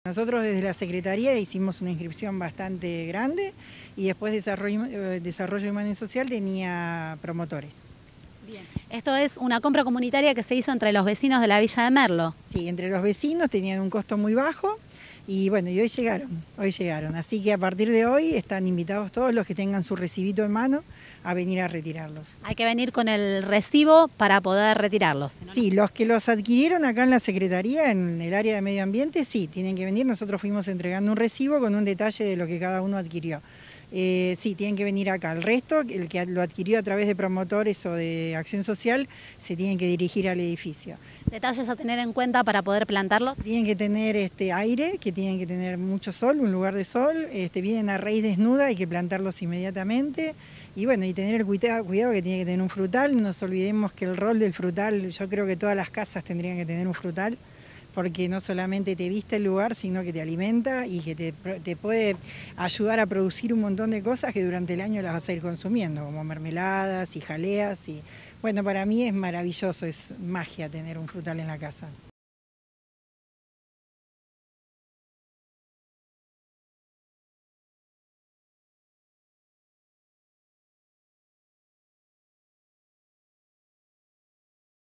ADQUISICIÓN DE FRUTALES
Unos mil árboles  llegaron al Municipio de la Villa de Merlo. Los vecinos que participaron para obtenerlos podrán acceder a diferentes clases, como ciruelos, durazno, peras, membrillo, almendros y manzano.